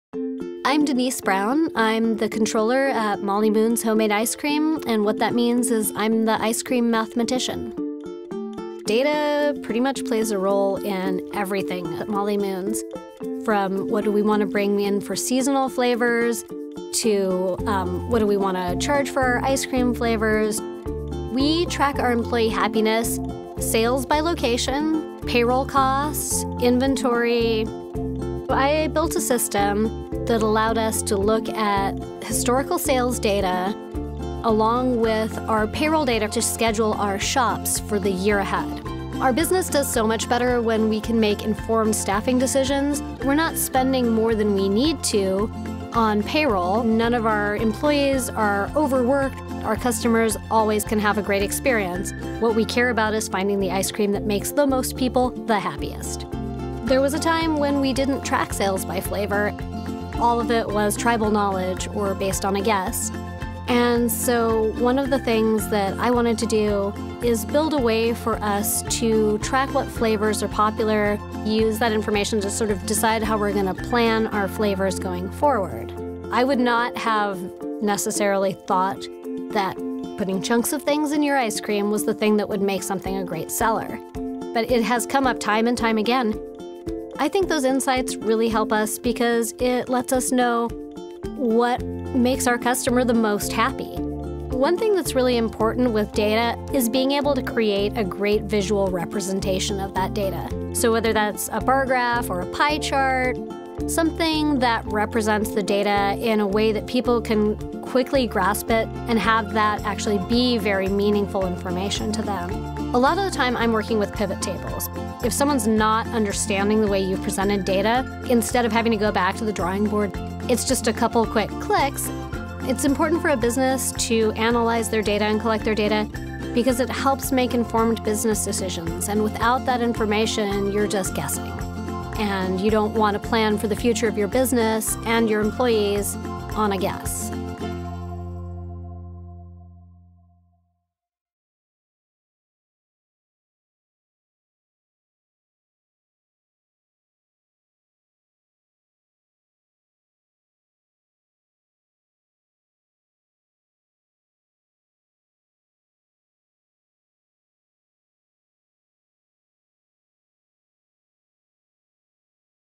How Molly Moon's data analytics drives sales using Microsoft Excel
Denise Brown explains how she built her own data analytics platform with Office 365 to manage finances at Molly Moon's, a popular ice cream company.

Brown is the head of finance and a self-described “ice cream mathematician and vanilla bean counter.” Brown has built her own data analytics platform to strategically organize the data that contributes to happy employees and a winning brand.